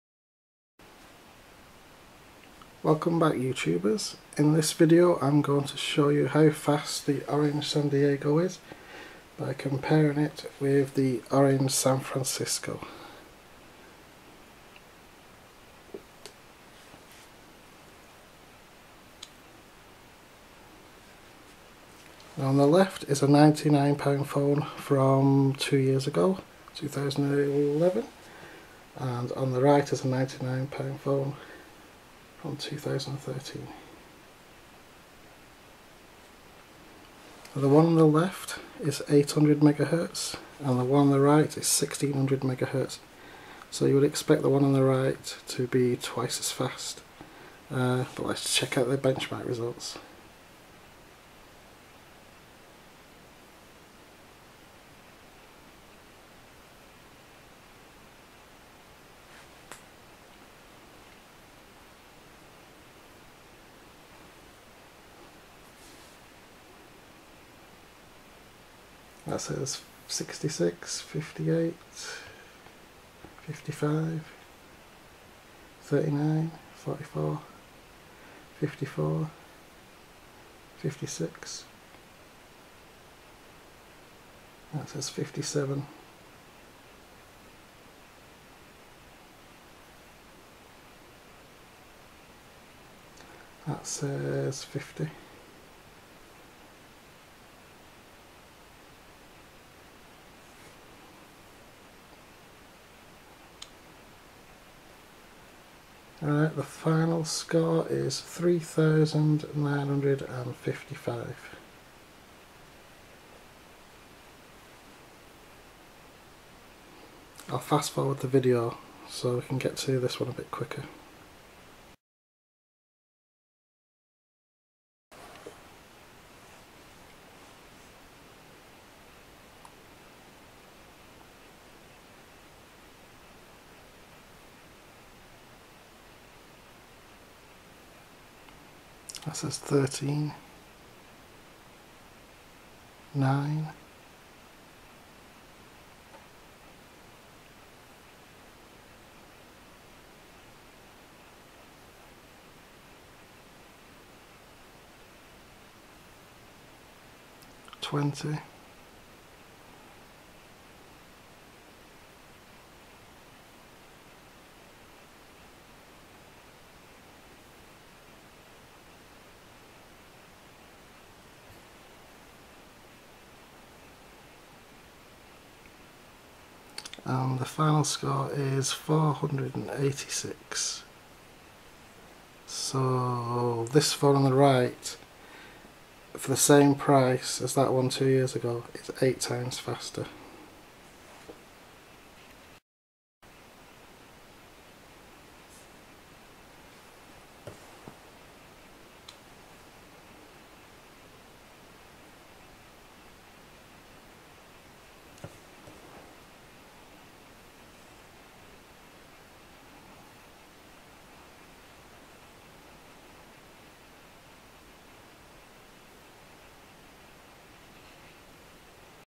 Orange San Diego vs. Orange San Francisco - Quadrant Benchmark Test
If you ever wondered how much faster the £99 (2013) Orange San Diego (AZ210A) would be over your £99 (2011) Orange San Francisco (ZTE Blade)? Up to 8x FASTER!